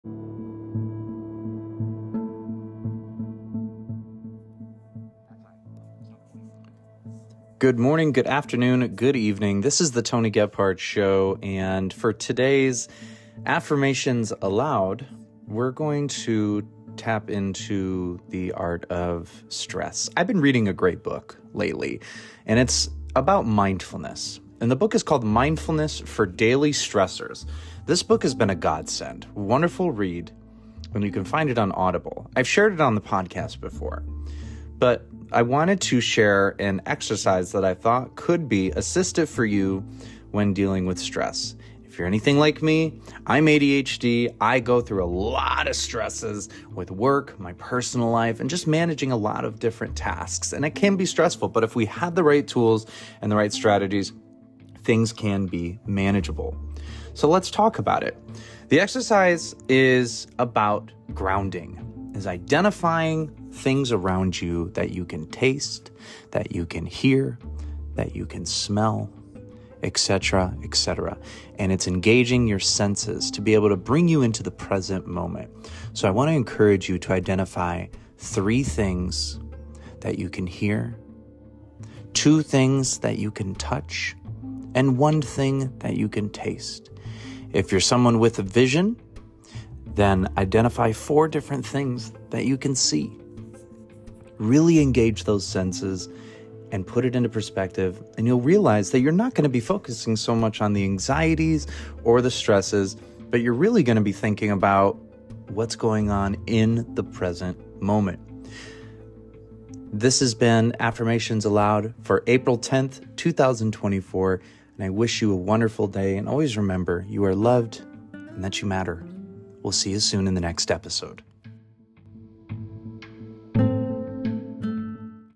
Affirmations allowed for April 10, 2024
Managing stress can be an extremely difficult thing for many, but I may have a few ideas to help you along with detangling those wires. Be sure to listen.